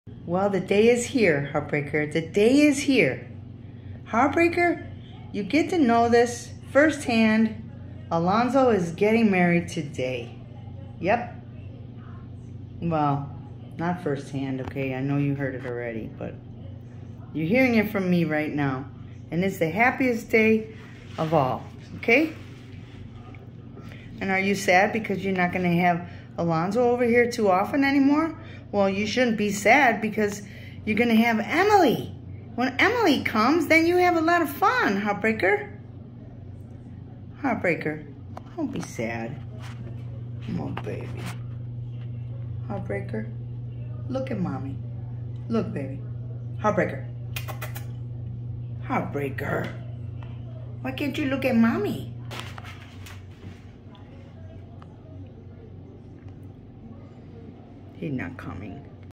Great Dane Love He Not Coming
Heartbreaker is sad again bcuz he doesn't get to go to Alonzo and Emily's wedding.